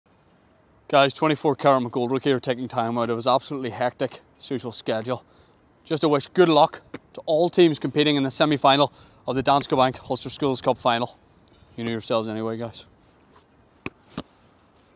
Danske Bank Ulster Schools' Cup 2022/2023 – Shane Todd (IV)
Mike McGoldrick (AKA Shane Todd) is back, providing his unique inspiration ahead of the Danske Bank Ulster Schools' Cup semi-finals 🏉 🏆